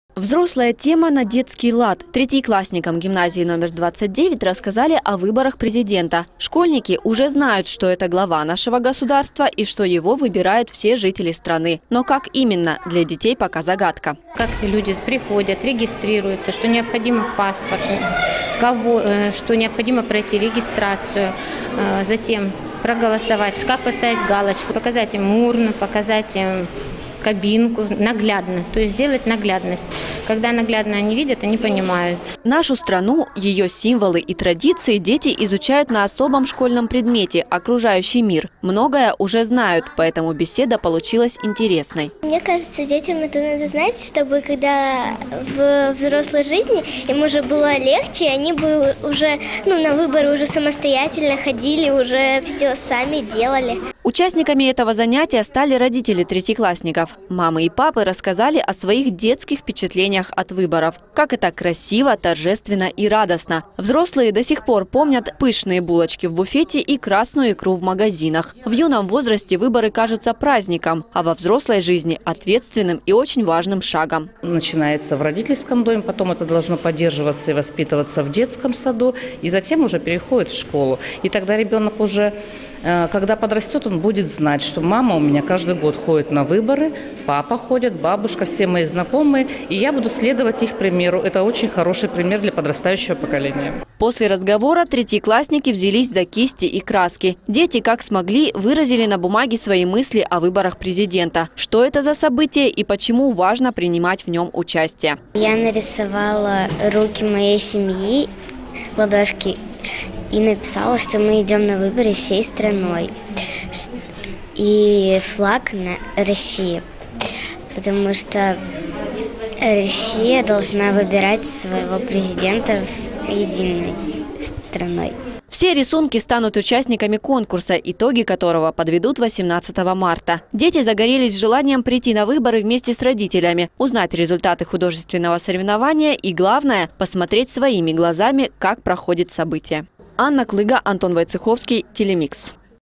Третьеклассники Уссурийска пойдут на выборы Президента
Третьеклассники пойдут на выборы Президента, за компанию с родителями. Так решили сами дети после рассказа взрослых о голосовании. На классном часе школьники поговорили об особенностях этого дня. Свои мысли и впечатления выразили в рисунке, каждый из которых отправится на конкурс.